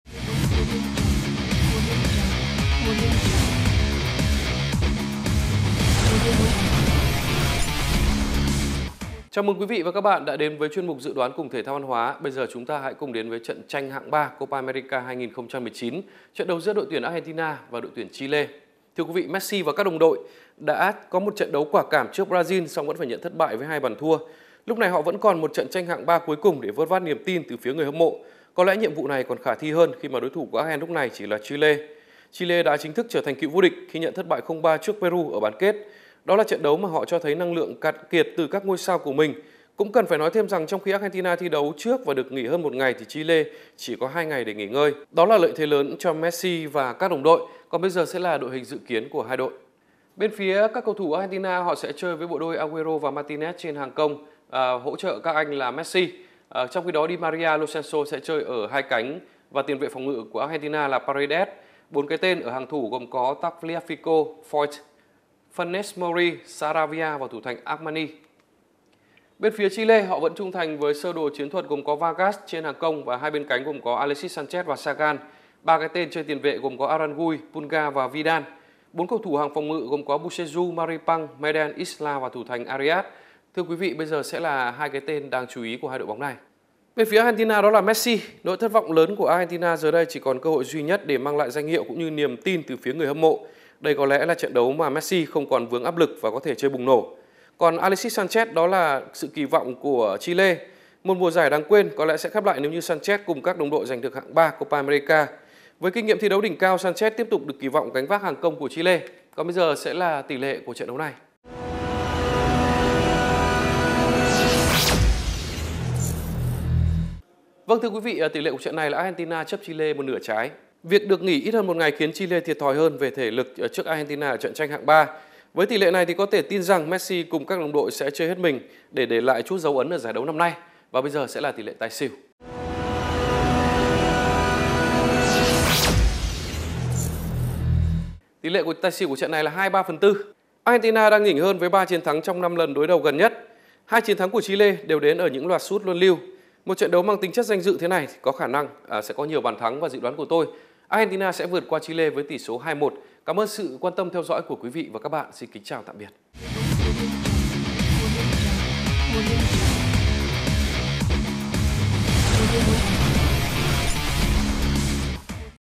[TRỰC TIẾP] Argentina vs Chile (2h00 ngày 7/7). Tranh hạng 3 Copa America. Trực tiếp K+PM, FPT Play
Argentina đã có 1 kỳ Copa America nữa đầy thất vọng và vị trí thứ 3 sẽ là sự an ủi cho Messi và đồng đội nếu đánh bại Chile trong trận đấu cuối cùng.
Mời quý vị cùng theo dõi chương trình nhận định và dự đoán kết quả trận tranh hạng 3 Copa 2019 giữa Argentina vs Chile do Thể thao & Văn hóa thực hiện.
Lịch thi đấu và truyền hình trực tiếp Copa America 2019:
2h00 ngày 7/7 - Tranh hạng 3: Argentina vs Chile (K+PM, FPT Play)
3h00 ngày 8/7 - Chung kết: Brazil vs Peru ( K+PM, FPT Play)
Link xem trực tiếp trận tranh hạng 3 Copa 2019 giữa Argentina vs Chile: